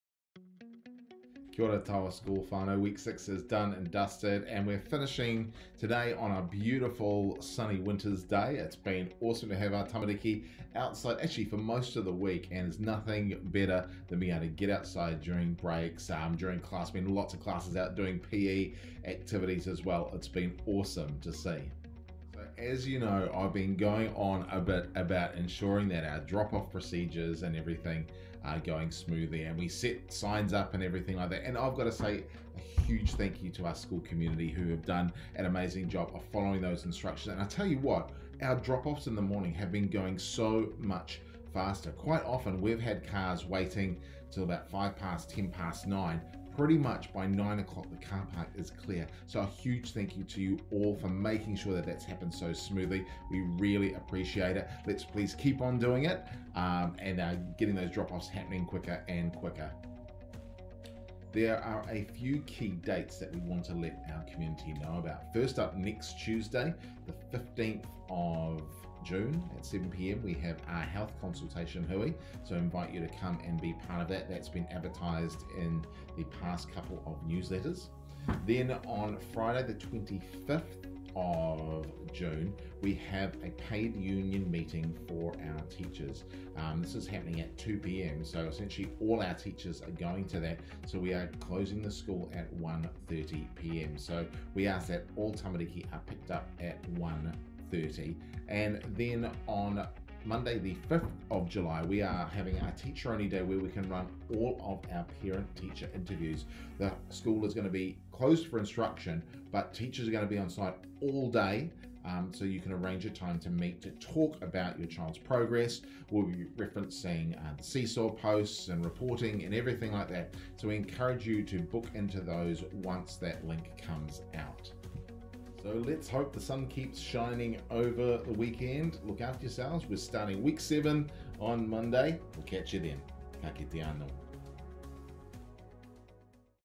2021 T2W6 Bulletin
This week we are talking about:
- Morning Drop-Off Thanks
- Health Consultation Hui (15/06/21)
- NZEI PUMs (25/06/21
- Teacher Only Day for Parent/Teacher Interviews (05/07/21)